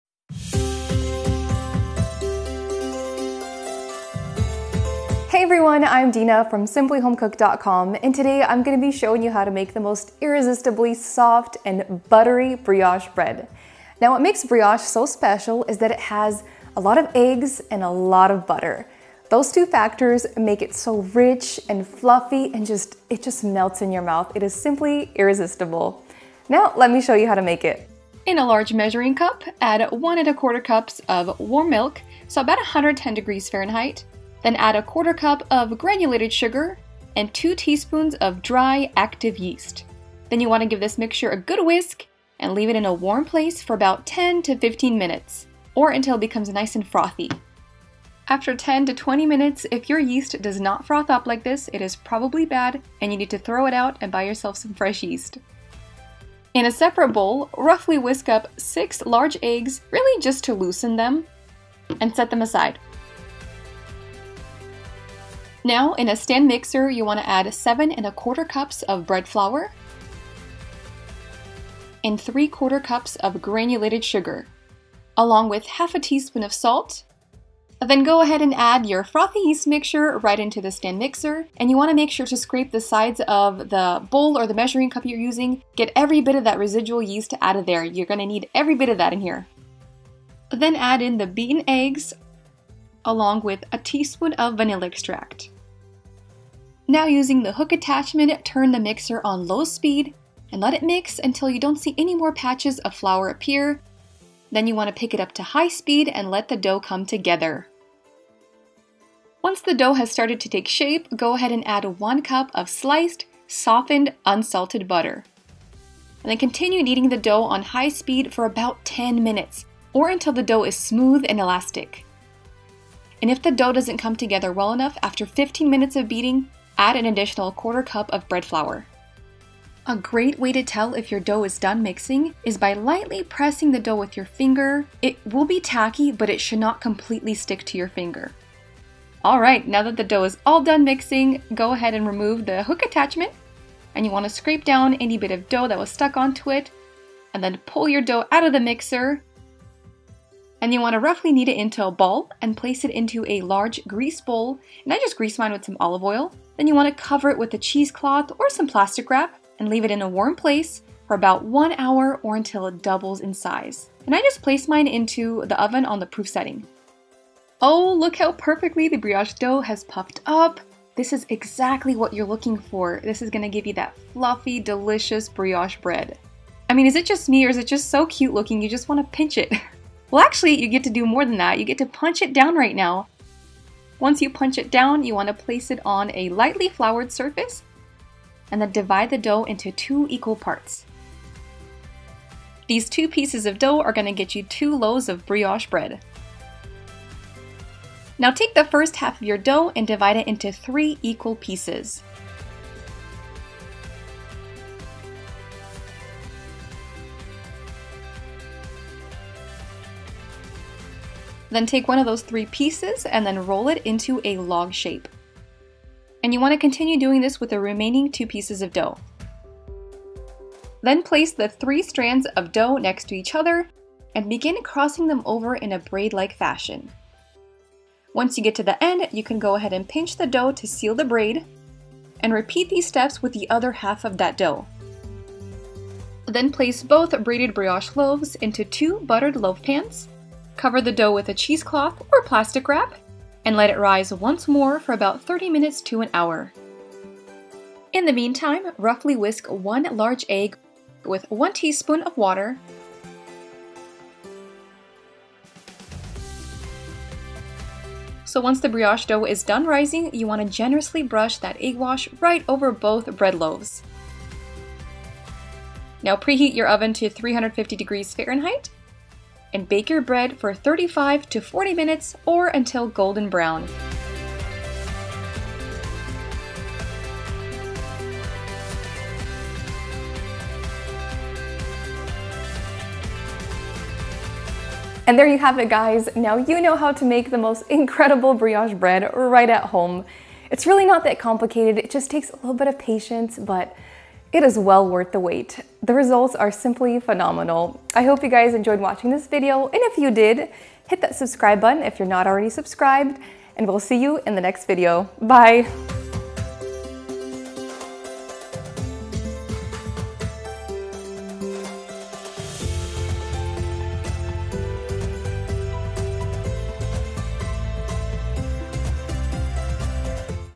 How to make Fluffy Brioche Bread
This brioche bread recipe is so fluffy it practically melts in your mouth. Serve it warm or toasted with salted butter and honey on top!

🥑🍓🥦🍉🍋 INGREDIENTS 🥔🥚🍫🧁🎂🍰

* 1 1/4 cup(296 ml) warm milk 110 degrees Fahrenheit
* 1 cup (200g) granulated sugar divided
* 2 tsp (7g) dry active yeast
* 7 1/4 cups (870g) bread flour spooned and leveled off with a knife
* 1/2 tsp(3g) salt
* 6 eggs (+1 egg for egg wash)
* 1 tsp (4.93ml)  vanilla extract
* 1 cup (227g)unsalted butter softened

➖Bosch mixer
➖Large Measuring Cup
➖Small whisk
➖King Arthur Bread Flour

OUR FAVORITE KITCHEN TOOLS
➖Measuring cups
➖Measuring spoons
➖Mini prep bowls
➖kitchen aid mixer
➖Bosch mixer
➖Nonstick pan
➖Cast Iron Pan

📃📄📃📄📃INSTRUCTIONS 📃📄📃📄📃📄

1. In a glass bowl or large measuring cup, combine milk, 1/4 cup sugar, and yeast. Leave this mixture in a warm place for about 20 minutes. Once it becomes frothy, give it a quick stir.
2. Place 7 1/4 cup bread flour, 1/2 teaspoon salt, and 3/4 cup granulated sugar in the bowl of a stand mixer. Then pour the warm yeast mixture into the bowl.
3. Crack the eggs into a bowl and give them a quick whisk just to loosen them. Add the beaten eggs and vanilla extract to the rest of the ingredients in the mixer bowl.
4. Using the hook attachment, beat the dough until it begins to come together.
5. Cut the 1 cup of unsalted softened butter into slices and add it to the dough. Continue kneading the dough at high speed for about 10 minutes or until the dough is smooth and elastic. If the dough isn't coming together well enough after 15 minutes, add an additional 1/4 cup flour.
6. Remove the brioche dough from the mixer and shape it into a ball. Place it into a bowl greased with oil. Then cover with a cheesecloth or plastic wrap and leave in a warm place for 1 hour or until it doubles or triples in size.
7. Once the dough has proofed, divide it into two equal pieces. These two halves will make two loaves of bread.
8. Starting with one of the two dough halves, divide that half into three equal balls. Roll those three balls into long strands.
9. Begin crossing over the three strands in a braid-like fashion. Once you get the end, pinch the dough to seal the braid. Repeat these steps with the other dough halve. (If you don't want to do a braid, just place the 3 dough balls on the loaf pan.)
10. Butter a loaf pan with unsalted butter and place the braided dough into the pan. Cover with a cheesecloth or plastic wrap and let it proof in a warm place for 1 hour or until it doubles in size.
11. Once the dough has proofed, brush with egg wash (1 beaten egg + 1 tsp water). Bake at 350 degrees Fahrenheit for 35-40 minutes until golden brown.

⏱ Time Stamps ⏱
Intro- 0:00
The Yeast Mixture- 0:28
The Brioche Dough Ingredients- 0:55
Mixing the Dough-1:36
First Proof- 2:18
Dividing the Dough- 3:05
Rolling the dough- 3:31
Braiding the Dough- 3:41
Second Proof- 3:56
The Eggwash- 4:07
Baking Your Brioche Bread- 4:24
Conclusion- 4:45